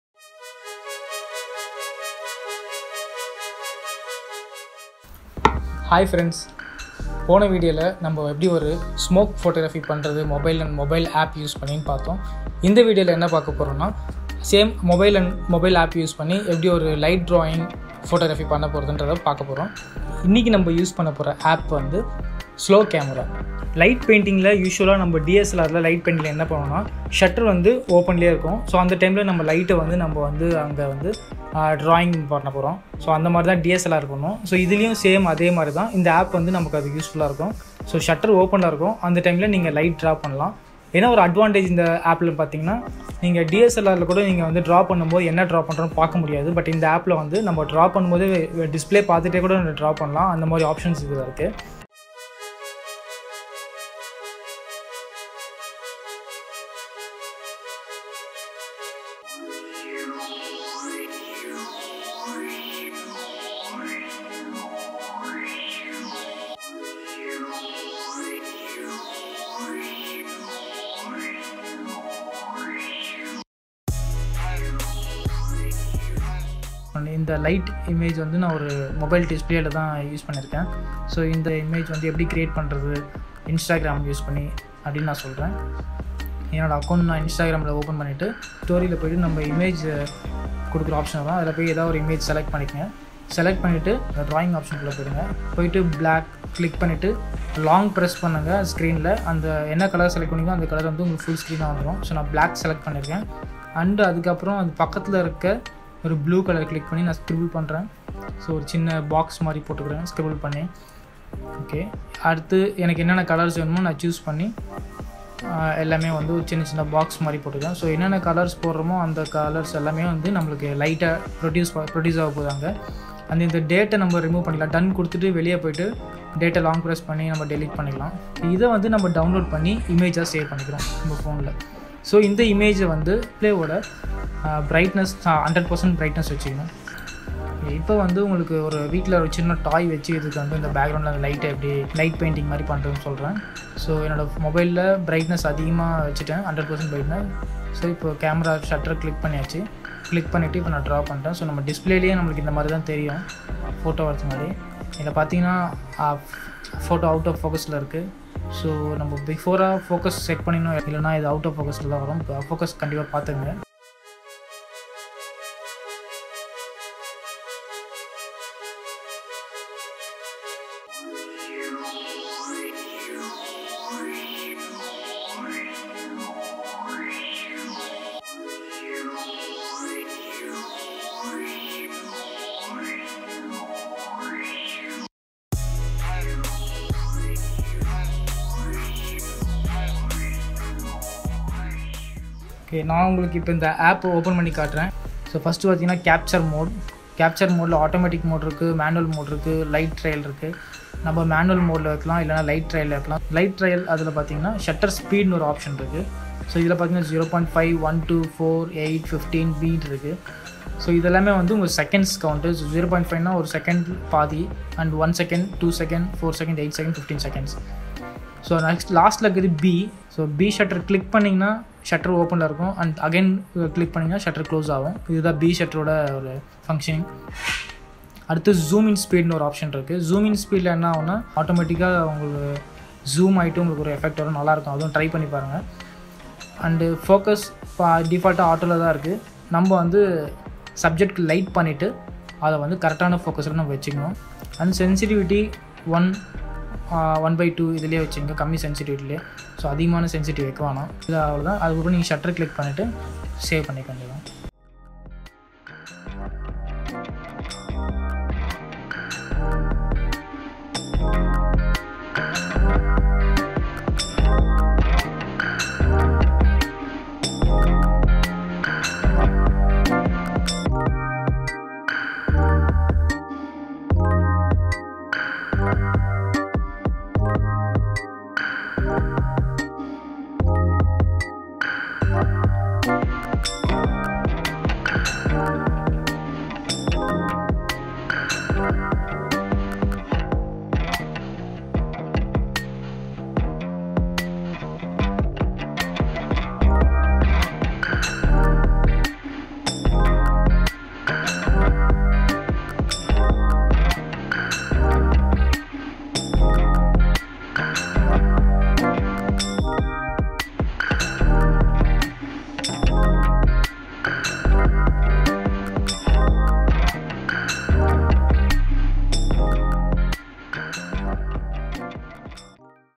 How to make - Light Painting - Tamil Photography - Mobile Photography - Quarantine Tasks
its an iPhone app

How to make Light Painting Photography using your Mobile Phone.
Interested in photographing using a mobile? This video will be useful to you. Instead of using a DSLR here, I tried using a mobile phone.